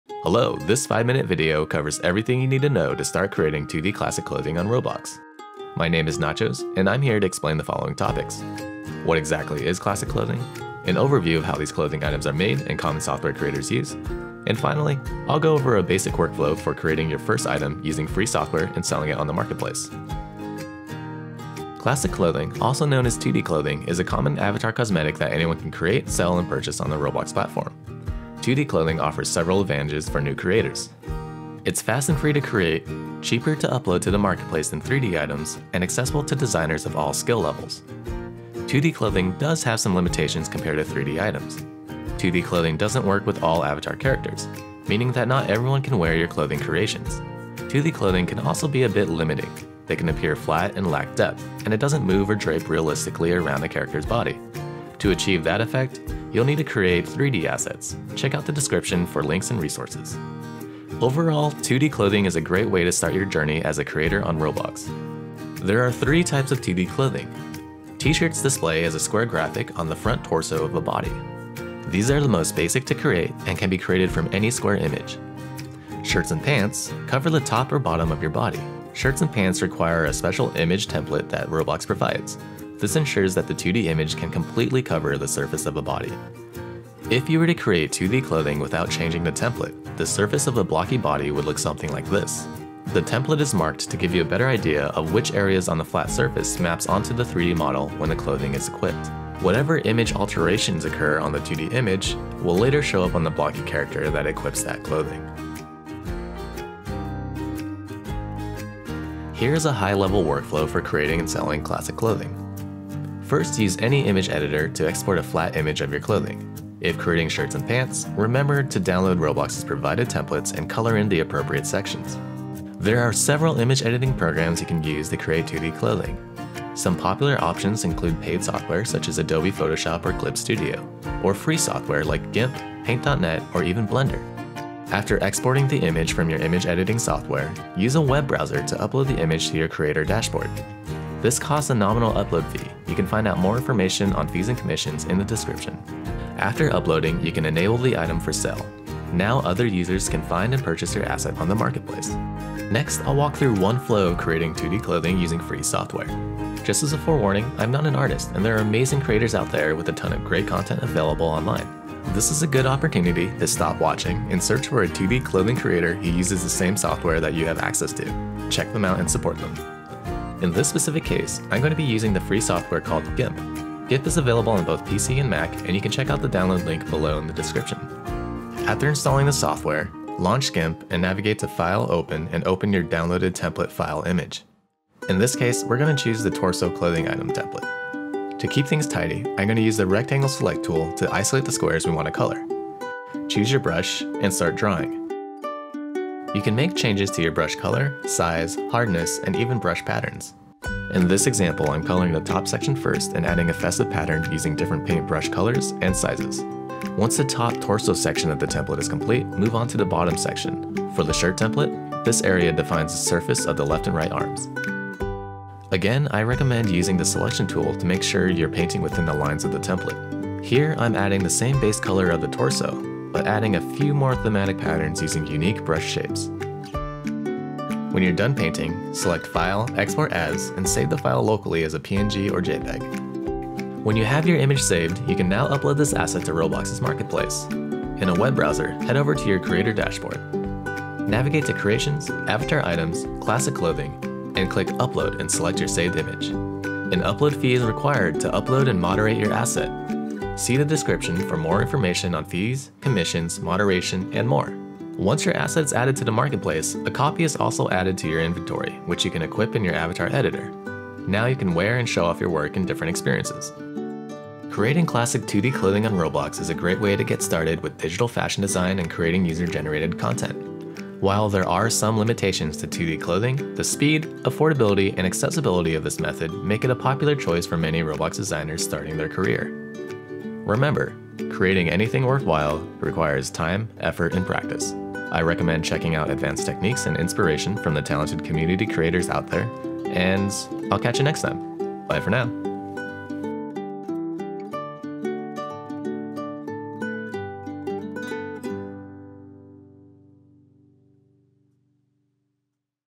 How to create and sell your first classic clothing item on Roblox
Learn how to create and sell your first classic clothing item on Roblox.

Classic clothing is the most basic and commonly created type of avatar item on Roblox! This video covers the basics of how to get started creating classic clothing as well as some tips on getting started with selling virtual items on the Marketplace.

Keep in mind that the final step of listing your asset on the marketplace requires an upload fee.

00:00 Introduction
00:21 What is Classic Clothing?
01:05 The 3 types of Classic Clothing
01:26 How templates Work
01:49 High level creation flow
02:04 Popular image editor software
02:18 Uploading to Roblox
02:32 Walkthrough
02:40 Check out other artists first!
03:07 Making a classic tshirt (GIMP)
03:53 Uploading to Roblox
04:24 Final thoughts